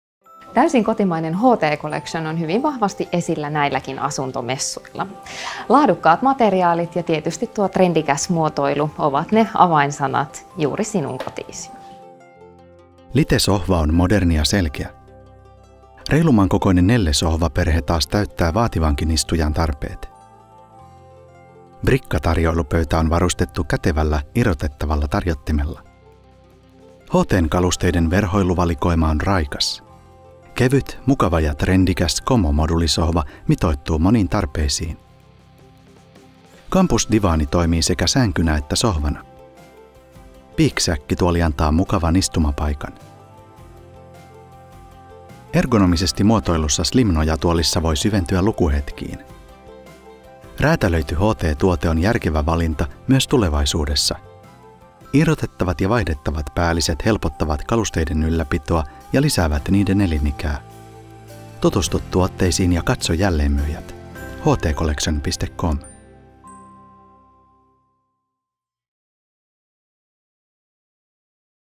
HT Collection - Innovatiivista muotoilua vuodesta 1959
Läpileikkaus Vantaan asuntomessujen 2015 tuotteista ja tiloista.